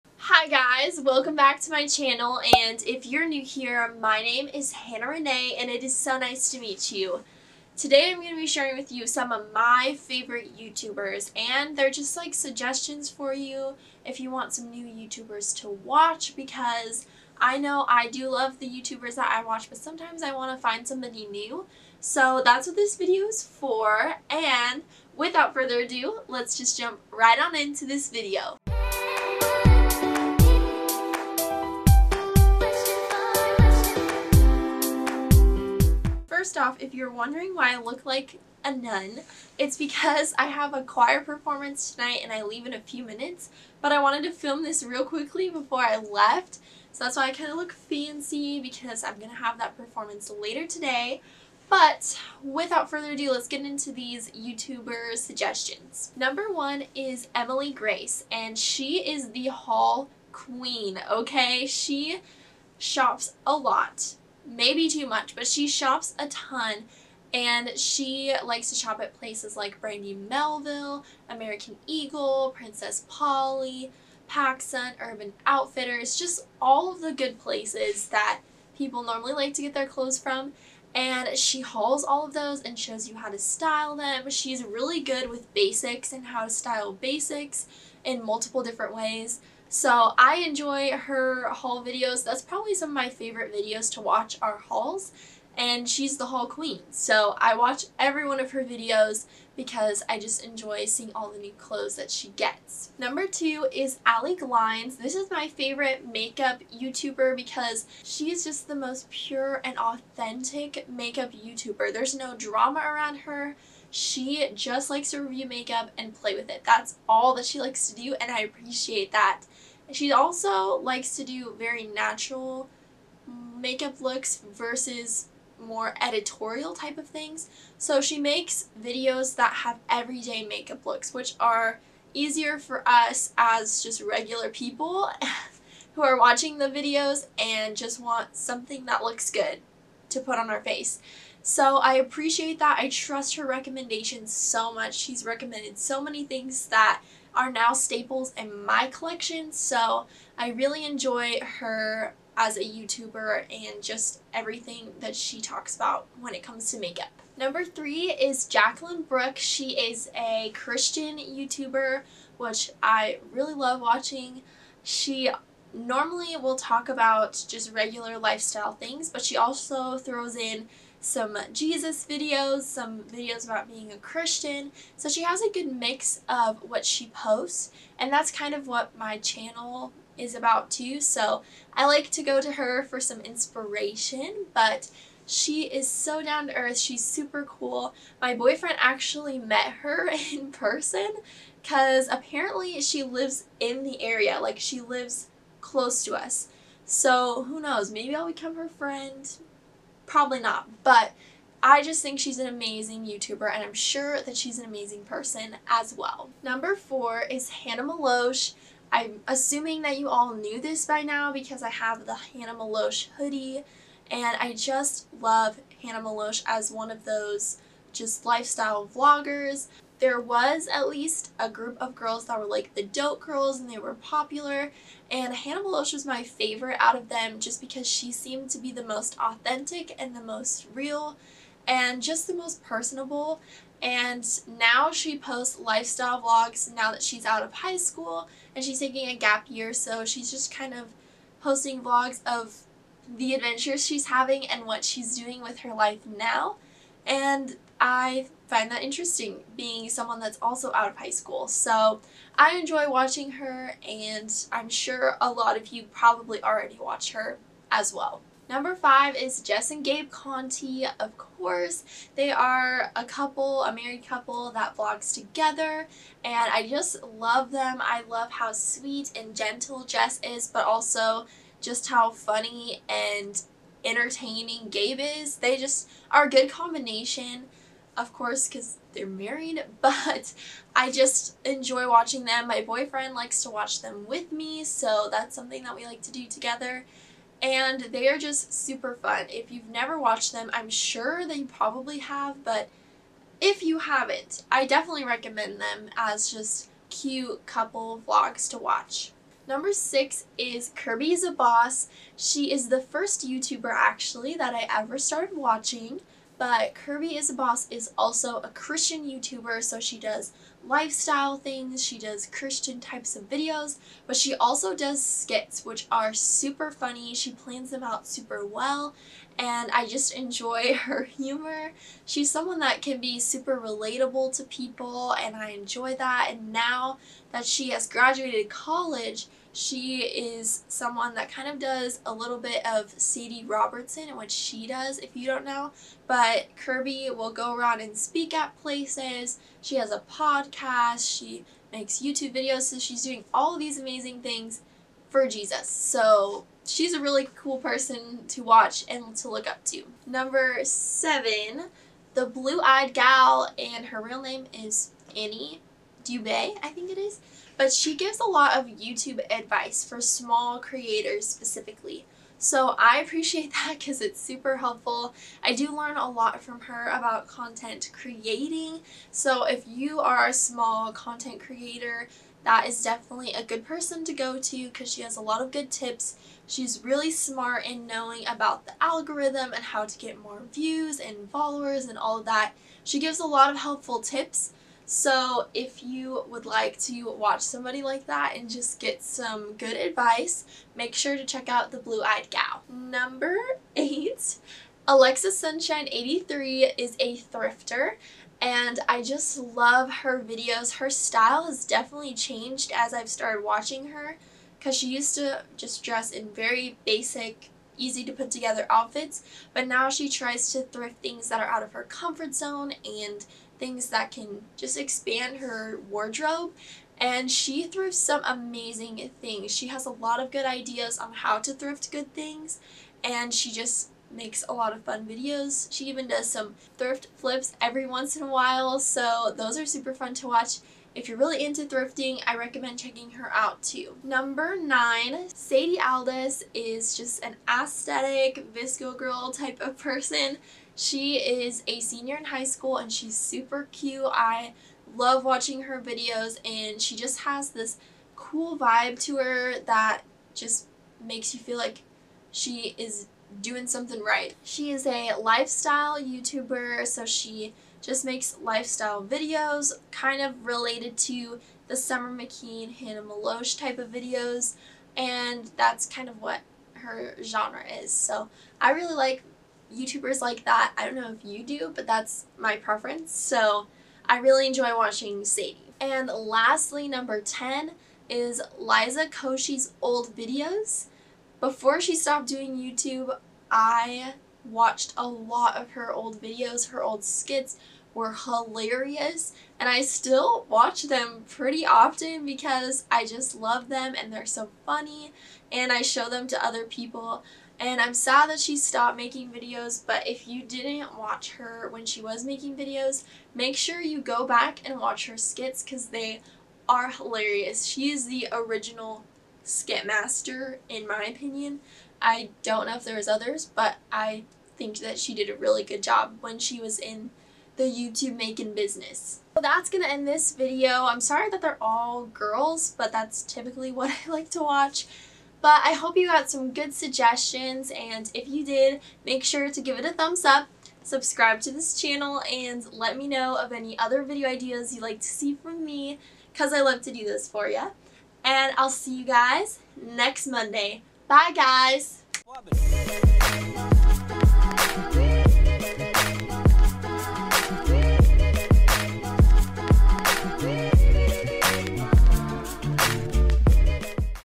10 YOUTUBERS YOU NEED TO WATCH//my favorite youtubers of all time
10 YOUTUBERS YOU NEED TO WATCH//my favorite youtubers of all time

Hi guys! I thought I would share some of my favorite YouTubers, and hopefully these content creators are suggestions for you guys to find new YouTubers to watch :) Enjoy!

Instagram & Tik Tok: hannahreneebanana
Poshmark: hannahrenee1319
VSCO: Hannah Renee